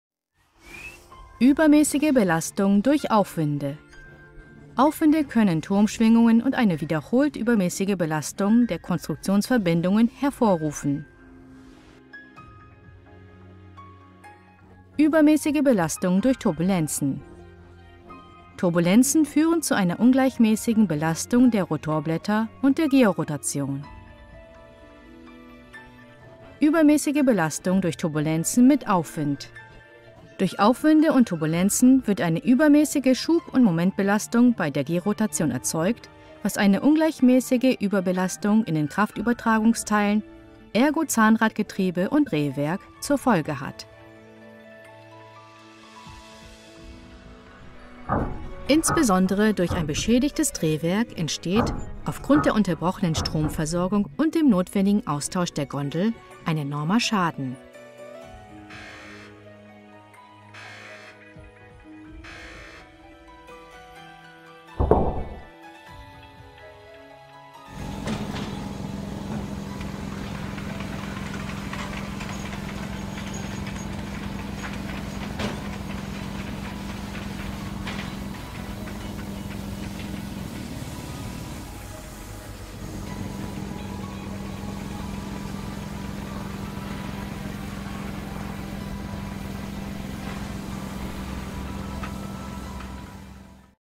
Spezialkran für Windkraftanlagen –CMFS-Ausrüstung für Nabtesco-Windkraftanlagen– Schadensvermeidung
Turbulenzen und Aufwinde verursachen Schwingungen im Turm, wodurch eine große Belastung an den Rotationskomponenten entsteht.